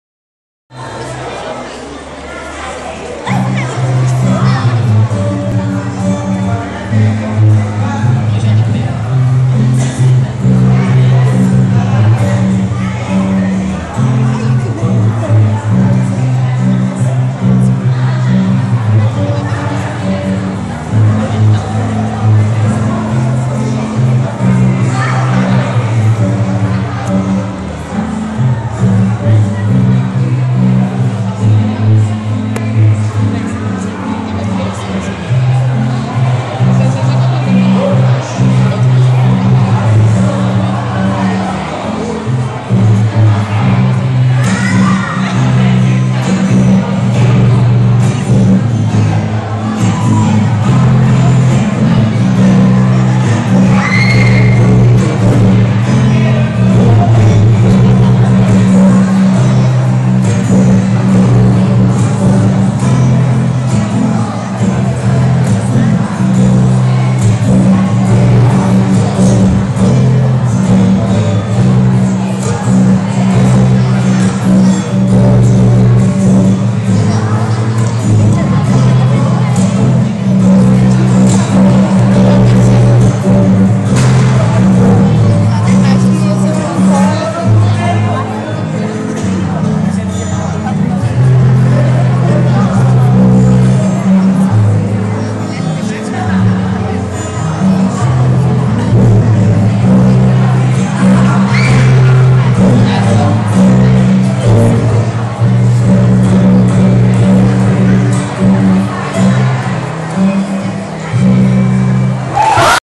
20121005_170210.mp4 - Olimpiarte - 2012
trabalho interdisciplinar com atividades em diversas áreas e um enfoque específico dentro do tema Jogos Olimpicos.